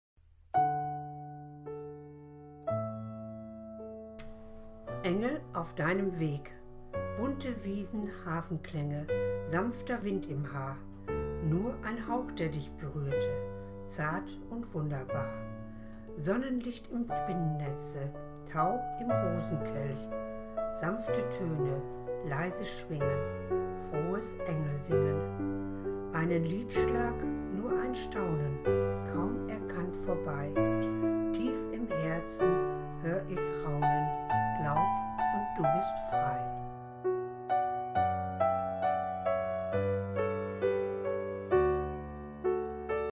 Engel sind auf deinem Weg, von Ingrid Heuwers
Gesprochenes Gedicht